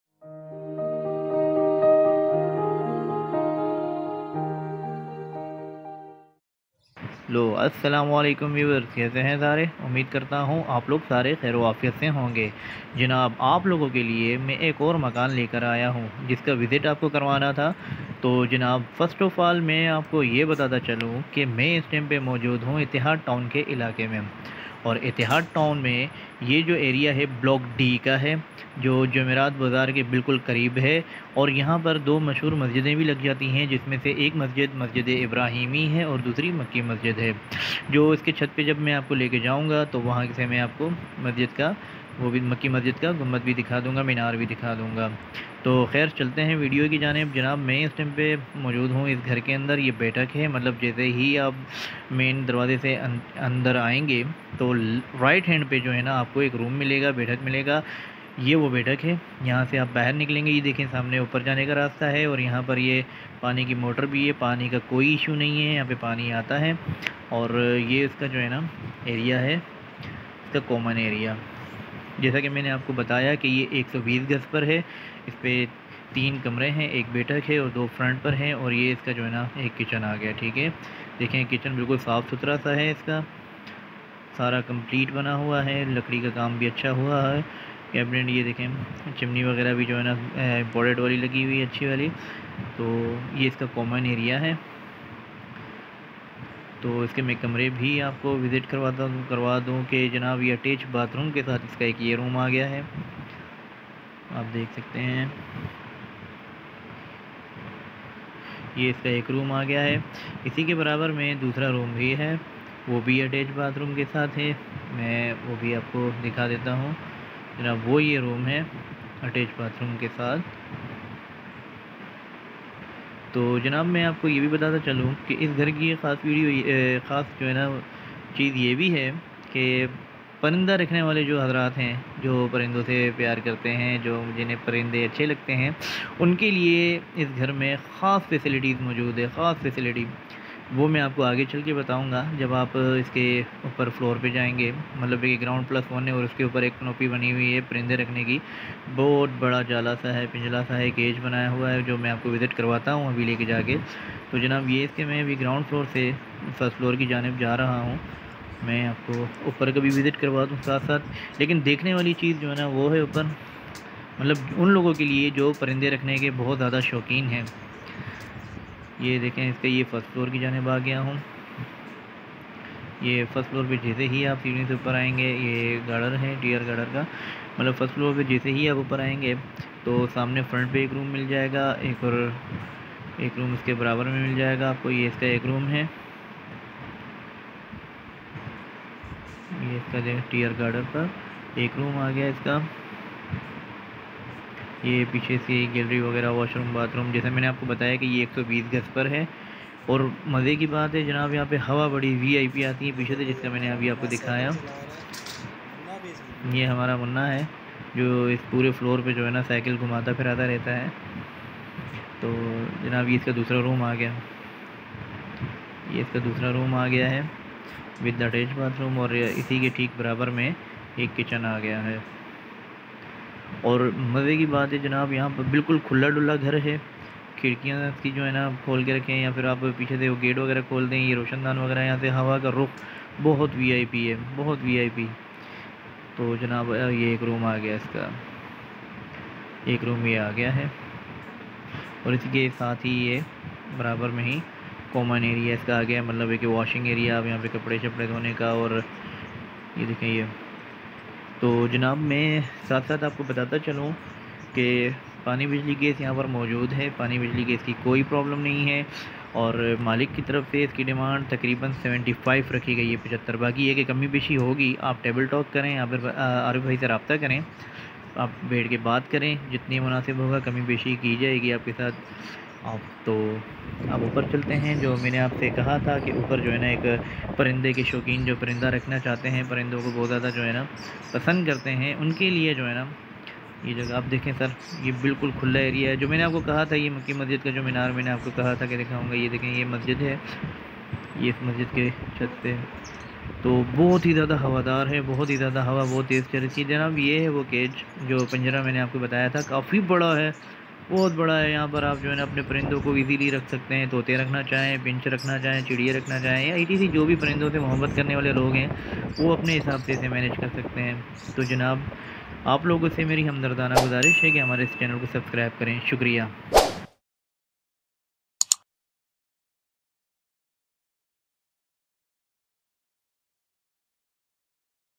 120 Gz Double Store House | Itehad Town Block-D
Itehad Town D-Block Near Jumerat Bazaar and Ibraheemi Masjid & Maqi Masjid.
120 Gz Double Store House and Parendon k Liya bht bara pinjra Jo 2nd Floor pr mojood Hy.
Demand : 75 Lakh, (Kami Peshi ki jay gi)
Muhammad Arif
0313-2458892